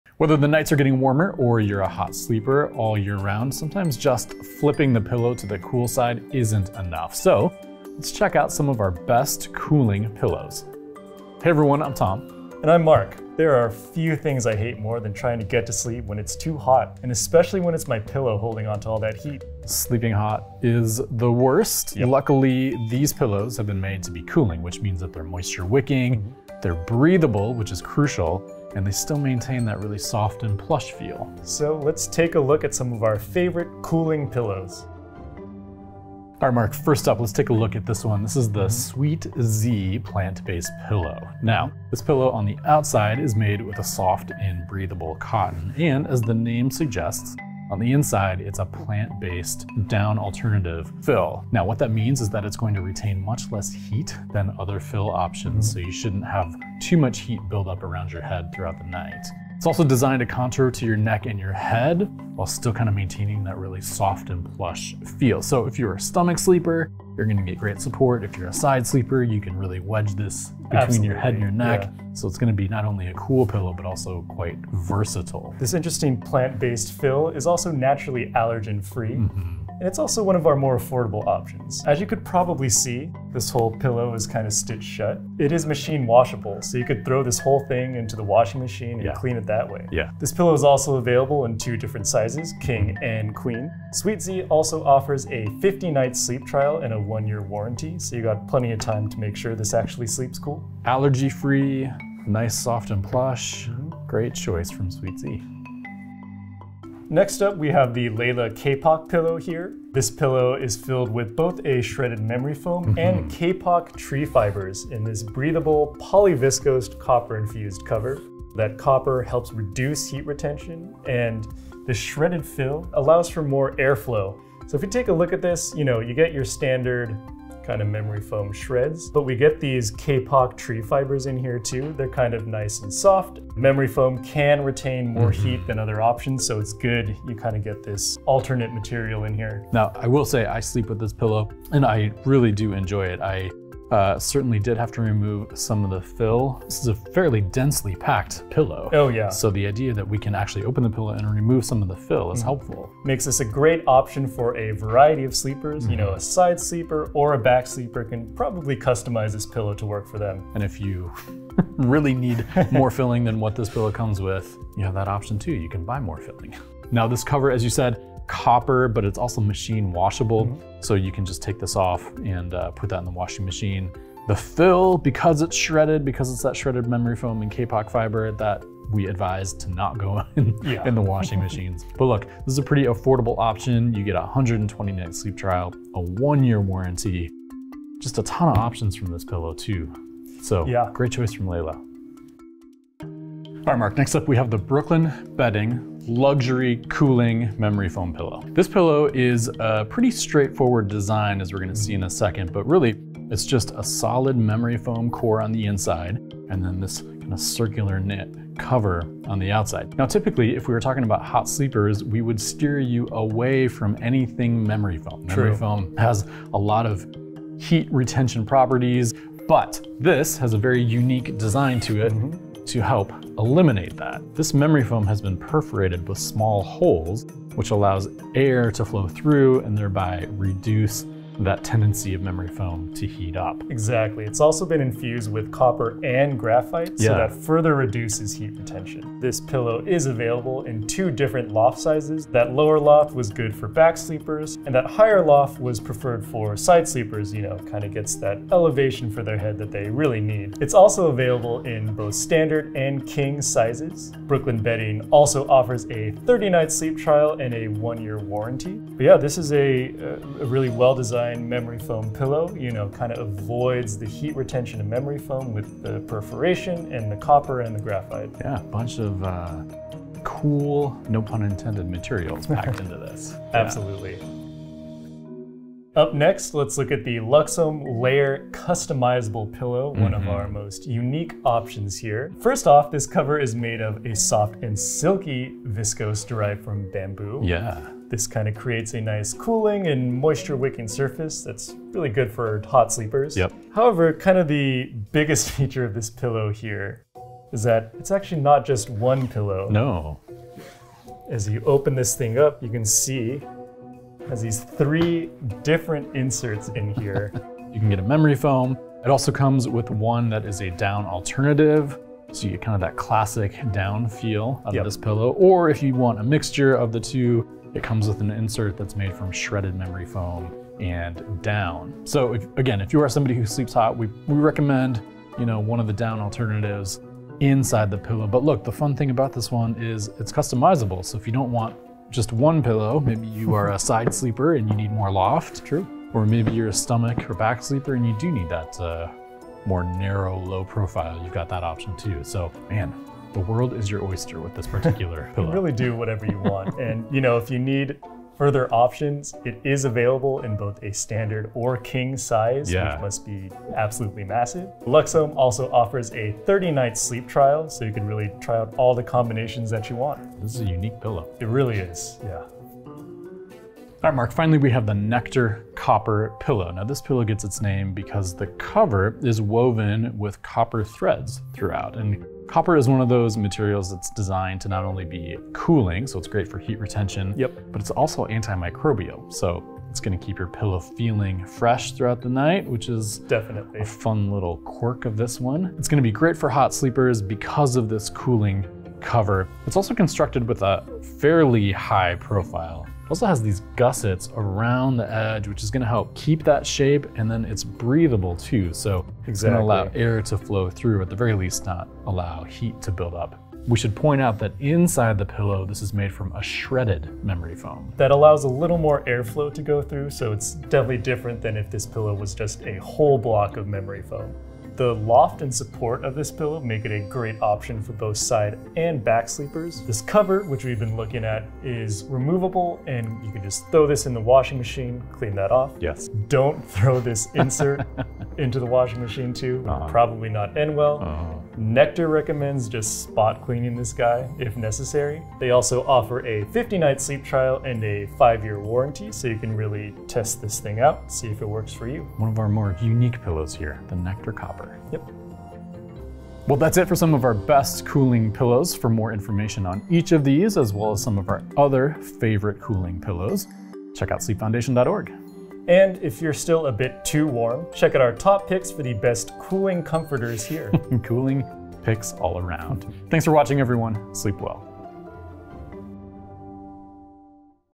Best Cooling Pillows - Our Top 5 Picks!
Click the links below for the most updated discounts on our favorite cooling pillows!

And check out Sleep Foundation’s page on our Best Cooling Pillows!

Whether the nights are getting warmer or you’re a hot sleeper year round, sometimes flipping the pillow to the cool side isn’t quite enough. Let’s check out some of the best cooling pillows of the year!

0:00 Introduction
0:35 Sweet Zzz Plant-Based Pillow
1:46 Layla Kapok Pillow
3:13 Brooklyn Bedding Luxury Cooling Memory Foam Pillow
4:35 Luxome LAYR Customizable Pillow
6:03 Nectar Copper Pillow